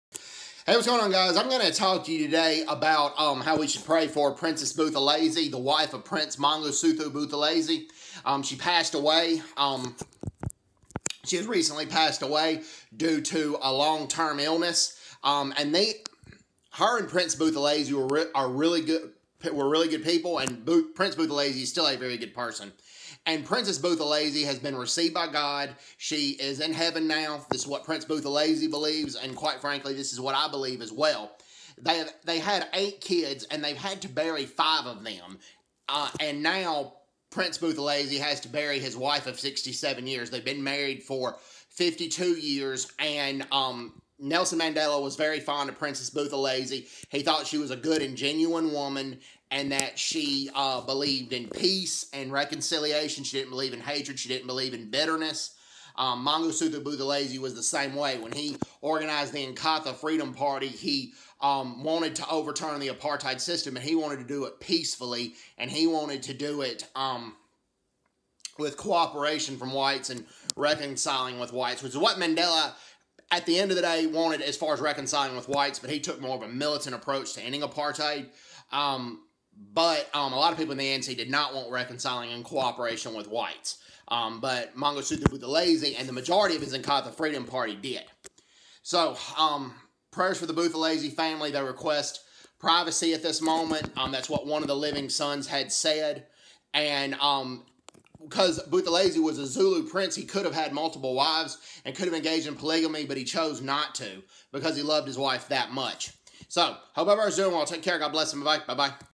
Prayers and condolences for the Buthelezi family
Princess Buthelezi has recently passed away and gone to heaven to be with God!!